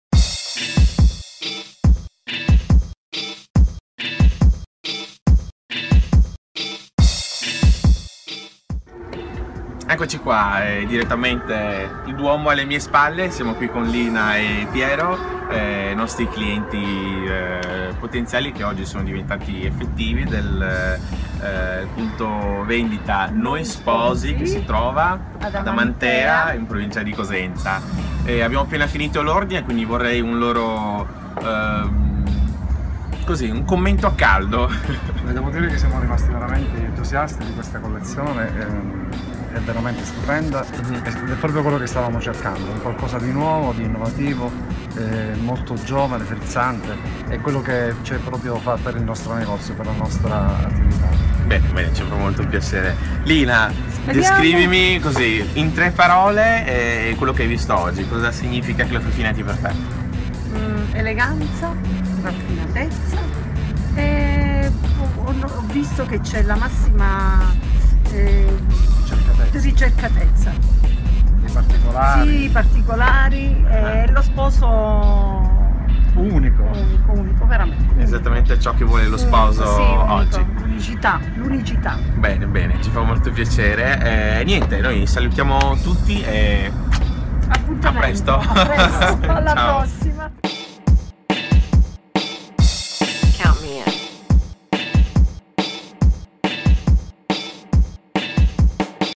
LINA E PIERO | NOI SPOSI | CLOFE FINATI BY ARCHETIPO
Seguite il viaggio attraverso Ànemos 2018, le nuove collezioni a firma Cleofe Finati, con le impressioni dei clienti!